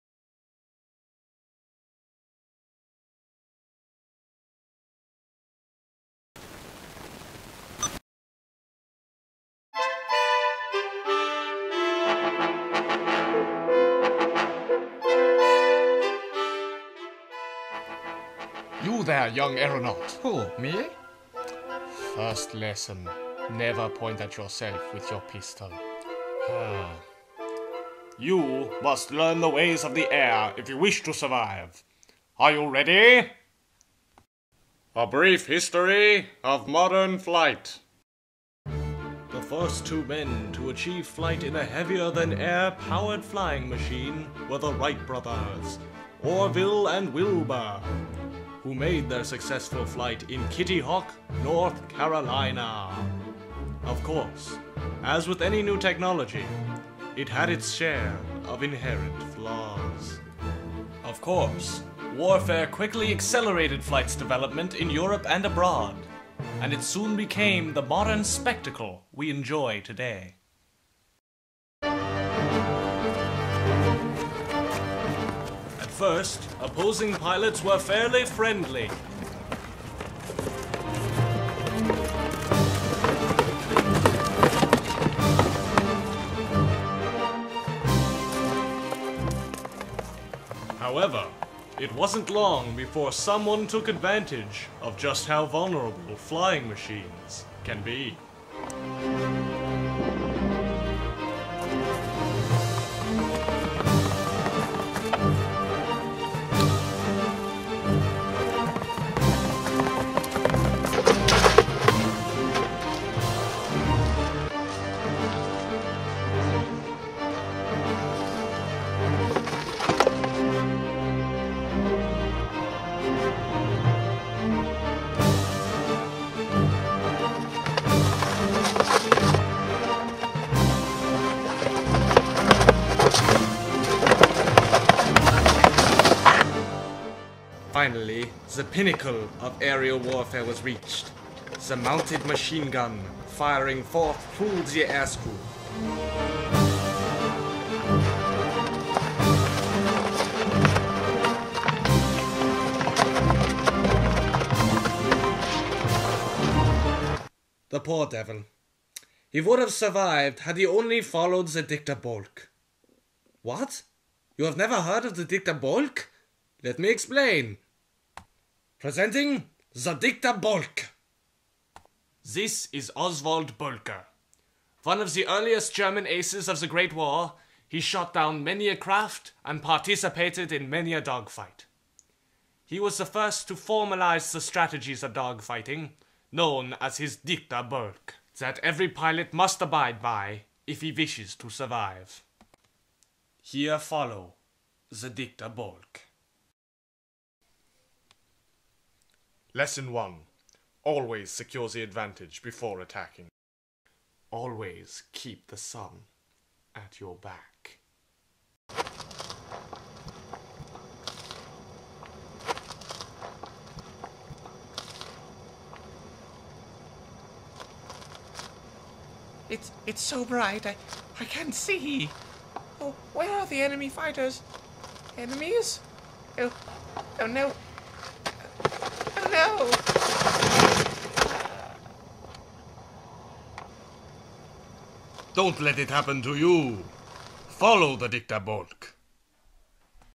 Pilot Training Film
We spent pretty much all day making those airplanes... this was for our history project on WWI air warfare.